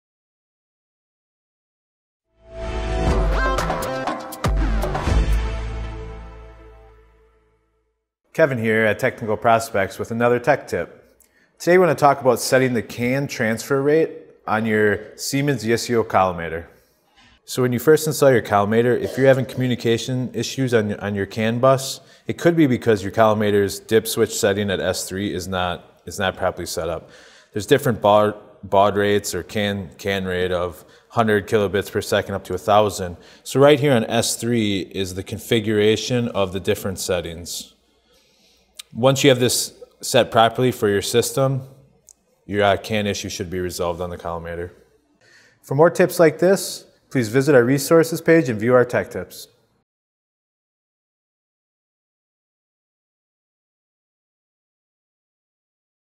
Tech Tip: Setting the CAN Transfer Rate on Siemens Collimator
In this tip, Kevin covers how to setup the CAN transfer rate on newer Siemens Radiography collimators. This particular collimator is on a Siemens Ysio X-ray system.